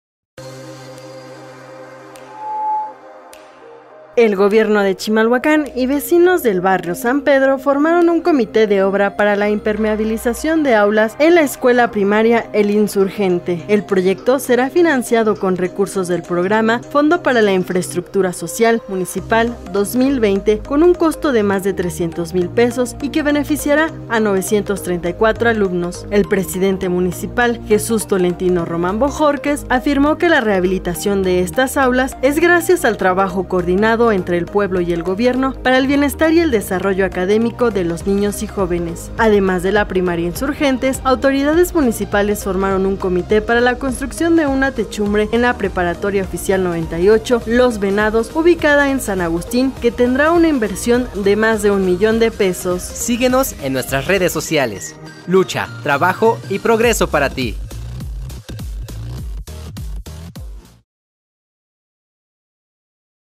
MEJORAMOS INFRAESTRUCTURA ESCOLAR EN SAN PEDRO
El gobierno de Chimalhuacán y vecinos de la zona comunal San Agustín, formaron un comité de obra para la impermeabilización de aulas en las en la escuela primaria “Insurgentes”, ubicada en el barrio San Pedro.

El proyecto será financiado con recursos del programa Fondo para la infraestructura Social Municipal 2020, con un costo de más de 300 mil pesos y que beneficiará a 934 alumnos.